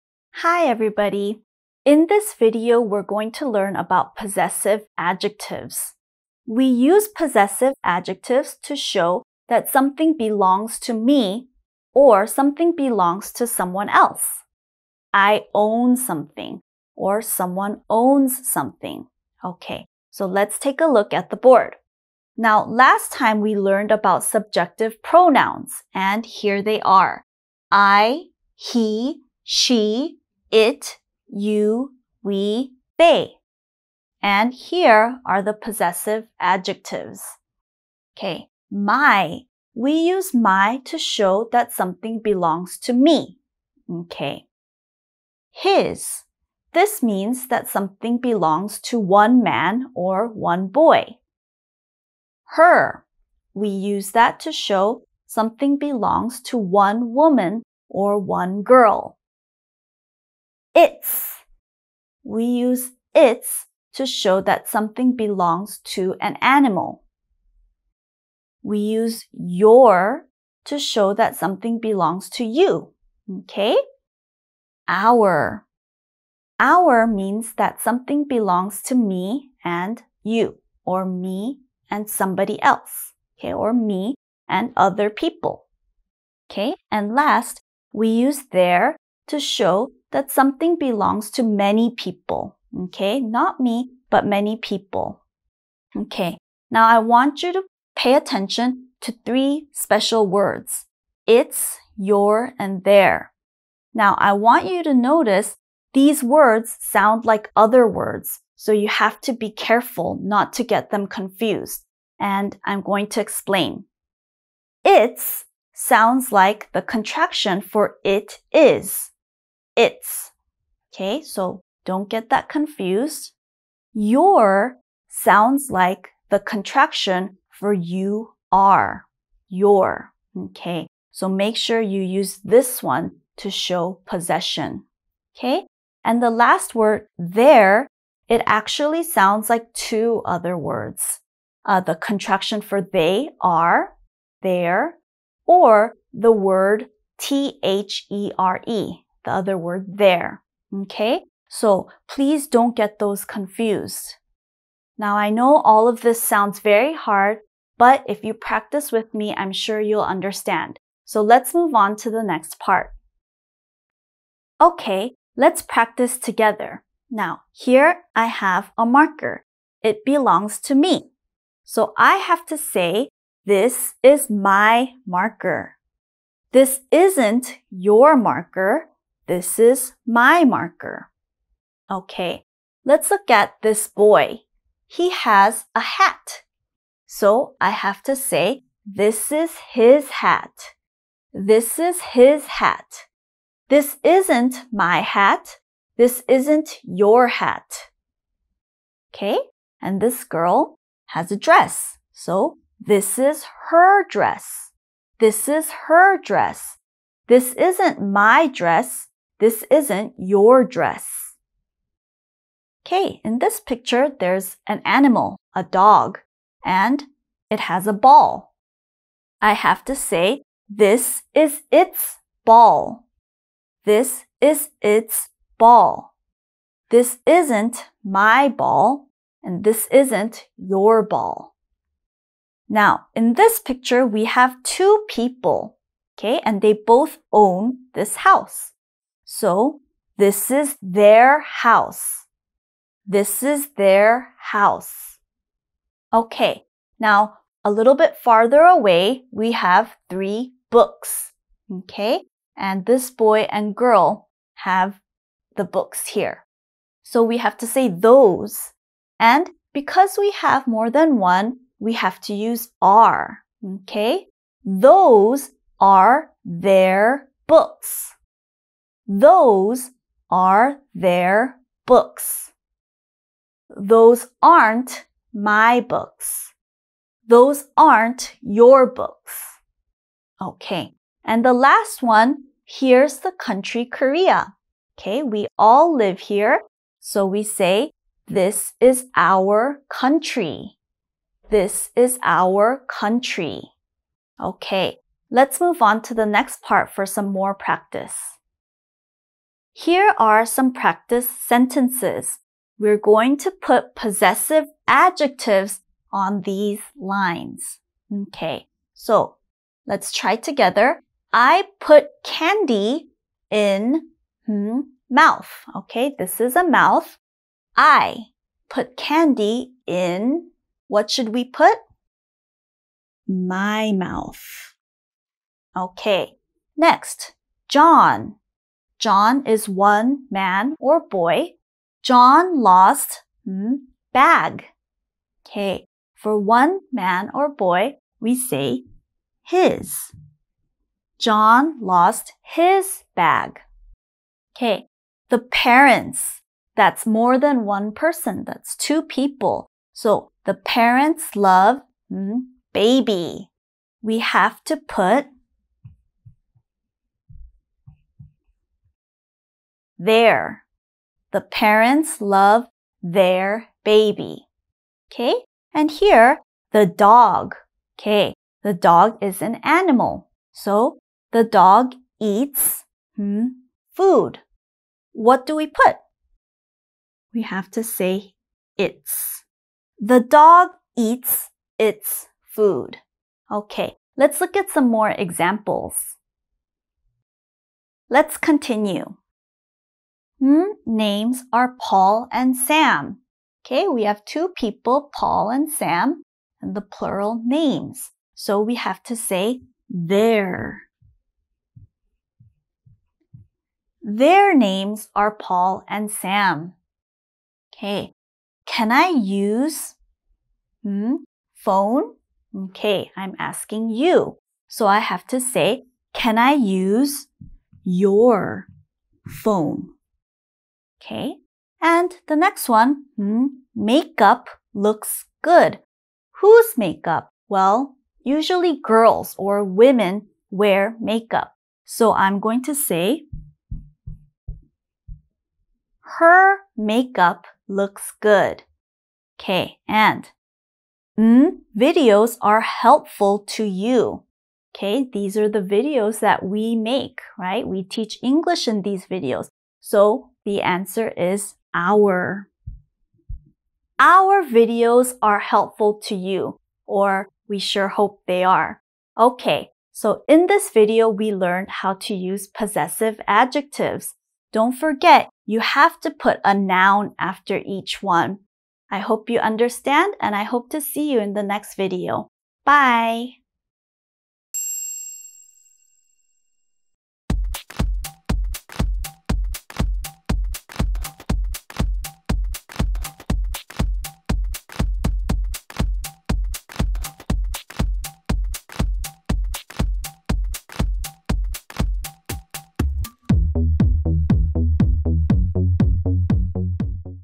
Learn Possessive Adjectives | Basic English Grammar Course
Esther teacher possessive adjectives in her Basic English Grammar Course.

Show Love and Support - Coffee For Teachers